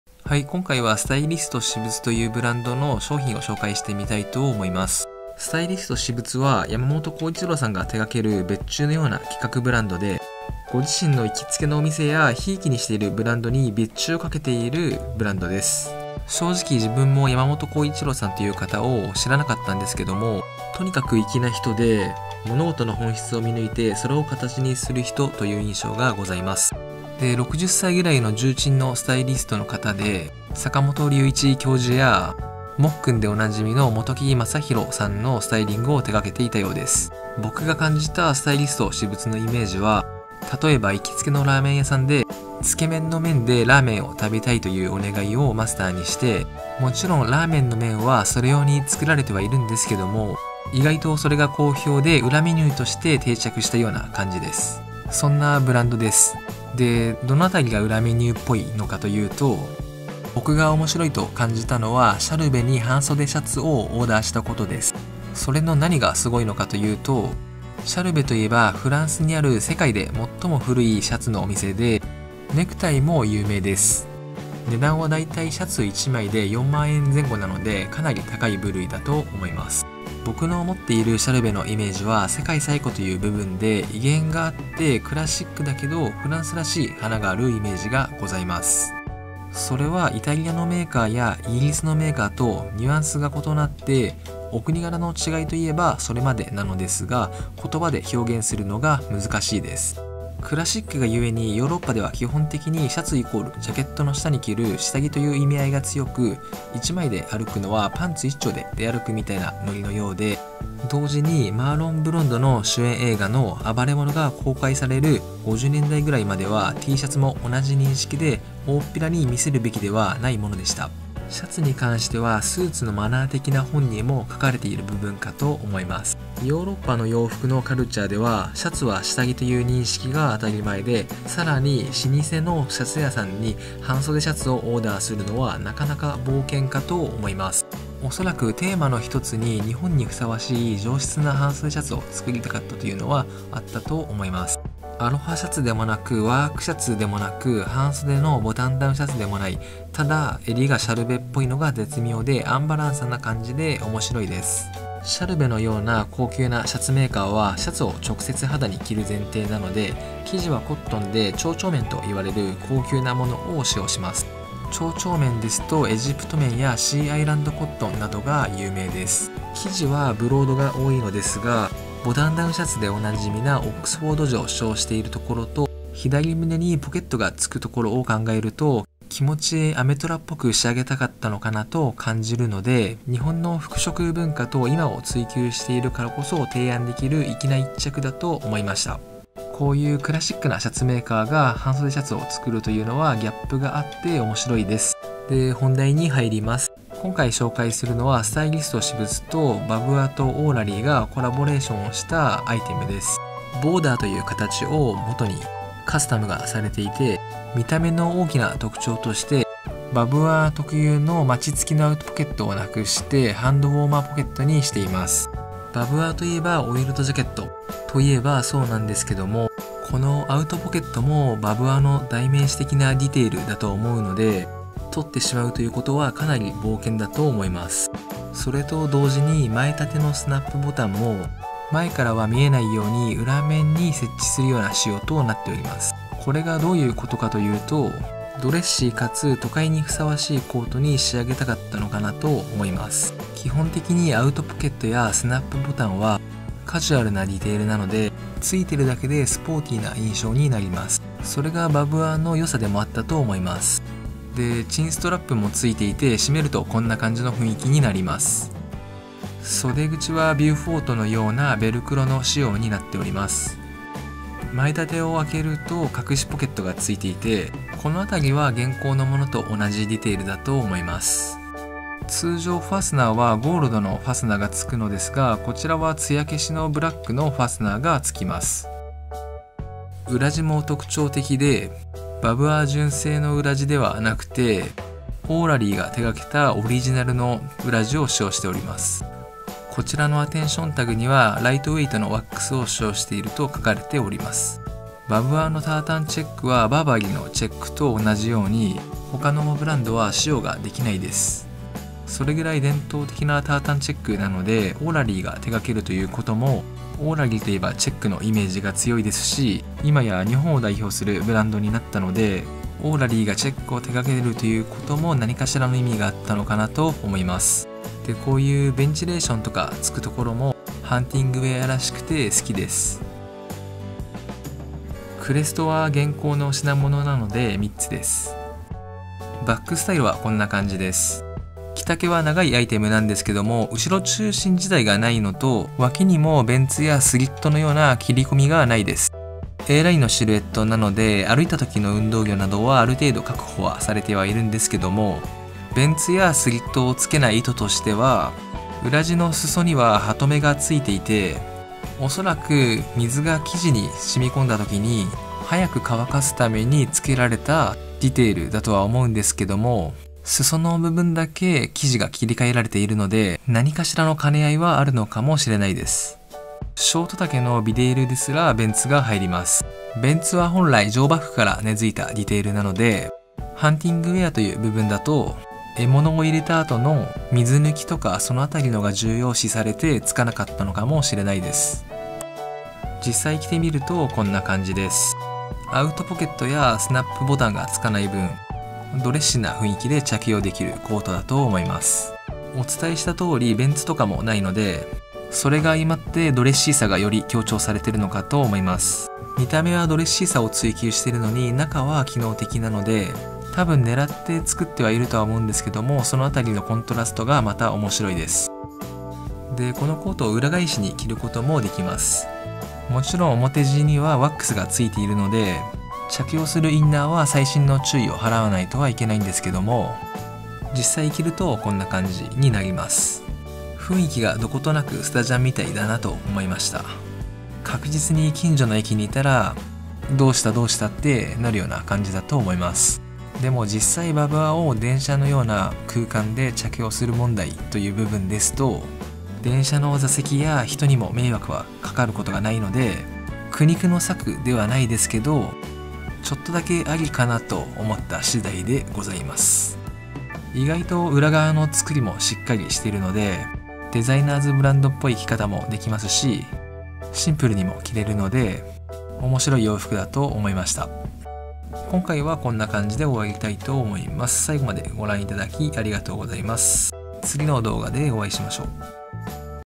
【名作裏バブアー】スタイリスト私物×barbour×AURALEEのBORDER
山本康一郎氏が手掛けるスタイリスト私物とバブアーとオーラリーのトリプルネームで出したボーダーの紹介です。個人的に好きなお店のH Beauty＆Youth で扱われていたアイテムです。ビンテージバブワーや現行のモデルにら無い都会的な雰囲気で着用できるロング丈のコートです。それぞれのブランドの考えが邪魔する事なく融合していて、バブワーも基本的な形は見てきましたが、今までにありそうでなかった所を上手くついていて名作になると思います。
リバーシブルではお洒落すぎて着用する機会が少ないので室内でこっそりコーディネートして着こなしてみました。